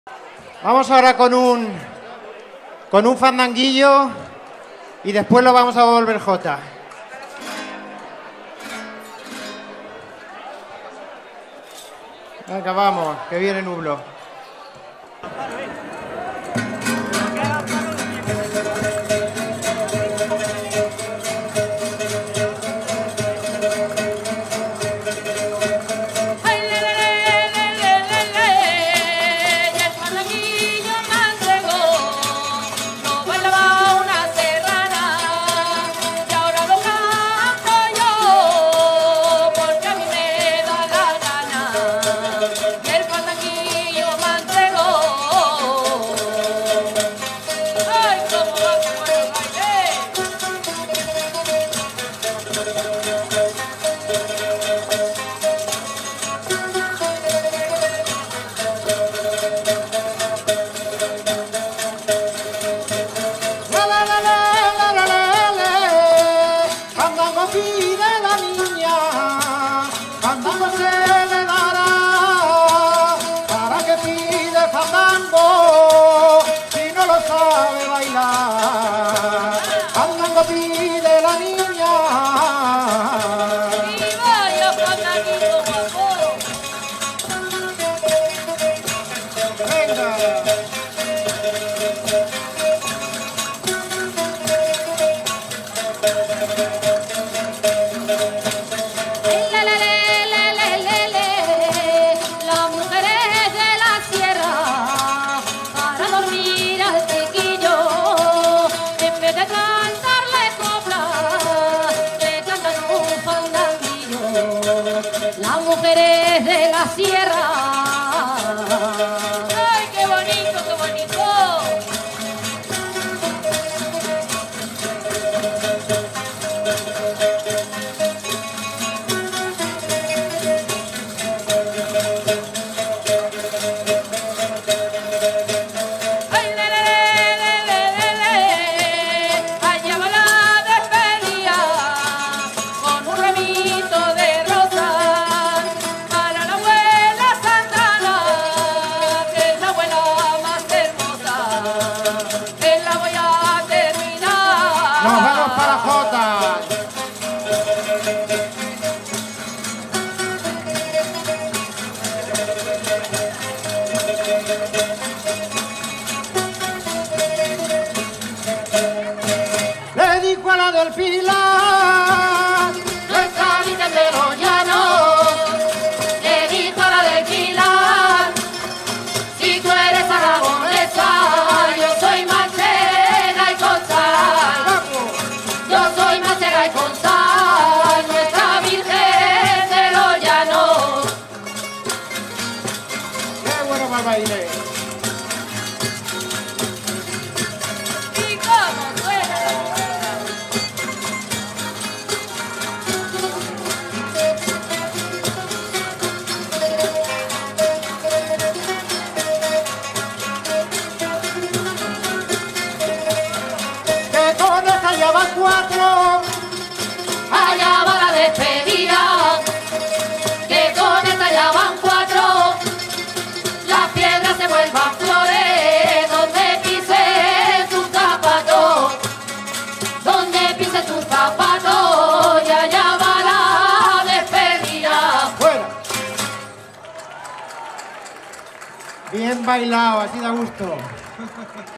XXX Encuentro de Cuadrillas Nerpio, 2022. Grupo de Folklore Abuela Santa Ana Fandanguillo-Jota
Si en Nerpio mi cuadrilla favorita es la Cuadrilla de Huebras, no lo es menos El Grupo de Folklore Abuela Santa Ana, aquí en Albacete.
Aquí os dejo este video, con un fandanguillo-jota, de su actuación en el XXX Encuentro de Cuadrillas, celebrado el pasado mes de febrero, en Nepio.